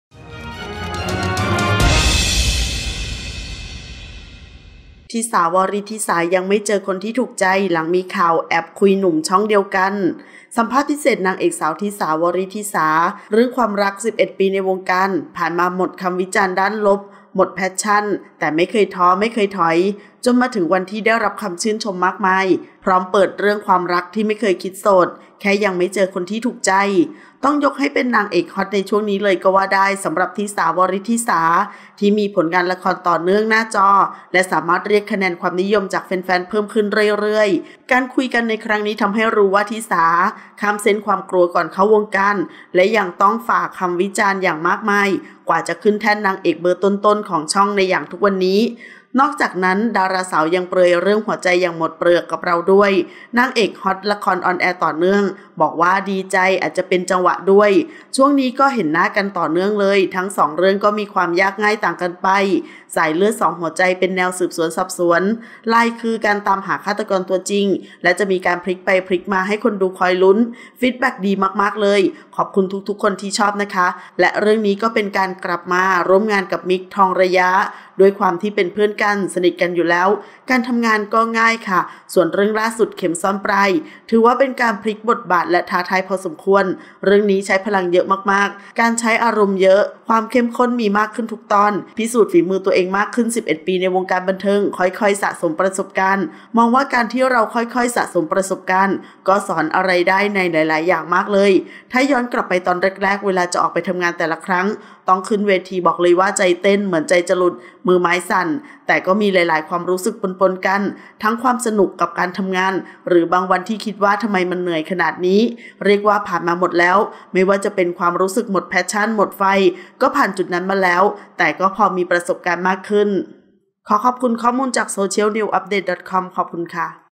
‘ฐิสา วริฏฐิสา’ ยังไม่เจอคนที่ถูกใจ หลังมีข่าวแอบคุยหนุ่มช่องเดียวกัน
สัมภาษณ์พิเศษ นางเอกสาว ฐิสา – วริฏฐิสา “เรื่องความรัก ลิ้มธรรมมหิศร” 11 ปีในวงการ ผ่านมาหมดคำวิจารณ์ด้านลบ หมดแพชชั่น แต่ไม่เคยท้อ ไม่เคยถอย

จนมาถึงวันที่ได้รับคำชื่นชมมากมาย พร้อมเปิดเรื่องความรักที่ไม่เคยคิด “โสด” แค่ยังไม่เจอคนที่ถูกใจ

ต้องยกให้เป็นนางเอกฮอตในช่วงนี้เลยก็ว่าได้ สำหรับ “ฐิสา – วริฏฐิสา ลิ้มธรรมมหิศร” ที่มีผลงานละครต่อเนื่องหน้าจอ
และสามารถเรียกคะแนนความนิยมจากแฟนๆ เพิ่มขึ้นเรื่อยๆ มาแรงขนาดนี้ พีพีทีวี นิวมีเดีย ไม่พลาดคว้าตัวมาพูดคุยกันอย่างเอ็กซ์คูลซีฟแน่นอน

การคุยกันครั้งนี้ทำให้รู้ว่า “ฐิสา” ข้ามเส้นความกลัวก่อนเข้าวงการ และยังต้องฝ่าคำวิจารณ์อย่างมากมายกว่าจะขึ้นแท่นนางเอกเบอร์ต้น ๆ ของช่องในอย่างทุกวันนี้

นอกจากนั้นดาราสาวยังเปลือยเรื่องหัวใจอย่างหมดเปลือกกับเราด้วย นางเอกฮอตละครออนแอร์ต่อเนื่อง
“ดีใจอาจจะเป็นจังหวะด้วย ช่วงนี้ก็เห็นหน้ากันต่อเนื่องเลยทั้งสองเรื่องก็มีความยากง่ายต่างกันไป “สายเลือดสองหัวใจ”

เป็นแนวสืบสวนสอบสวน ไลต์คือการตามหาฆาตกรตัวจริงและจะมีการพลิกไปพลิกมาให้คนดูคอยลุ้น ฟีดแบคดีมากๆเลย
ขอบคุณทุกๆคนที่ชอบนะคะ และเรื่องนี้ก็เป็นการกลับมาร่วมงานกับ “มิกค์” (มิกค์ ทองระย้า) ด้วยความที่เป็นเพื่อนกัน

สนิทกันอยู่แล้ว การทำงานก็ง่ายค่ะ ส่วนเรื่องล่าสุด “เข็มซ่อนปลาย” ถือว่าเป็นการพลิกบทบาทและท้าทายพอสมควร
เรื่องนี้ใช้พลังเยอะมากๆ จะการใช้อารมณ์เยอะ ความเข้มข้นมีมากขึ้นทุกตอน”

พิสูจน์ฝีมือตัวเองมากขึ้น 11 ปีในวงการบันเทิงค่อย ๆ สะสมประสบการณ์
มองว่าการที่เราค่อย ๆ สะสมประสบการณ์ก็สอนอะไรได้ในหลาย ๆ อย่างมากเลย ถ้าย้อนกลับไปตอนแรกๆ

เวลาจะออกไปทำงานแต่ละครั้ง ต้องขึ้นเวที บอกเลยว่าใจเต้นเหมือนใจจะหลุด มือไม้สั่น แต่ก็มีหลาย ๆ ความรู้สึกปนๆ กัน ทั้งความรู้สึกสนุกกับการทำงาน

หรือบางวันที่คิดว่าทำไมมันเหนื่อยขนาดนี้ เรียกว่าผ่านมาหมดแล้ว ไม่ว่าจะเป็นความรู้สึกหมดแพชชั่น หมดไฟ ก็ผ่านจุดนั้นมาแล้ว แต่พอมีประสบการณ์มากขึ้น

00:00 - Intro
00:10 - News
01:00 - Minute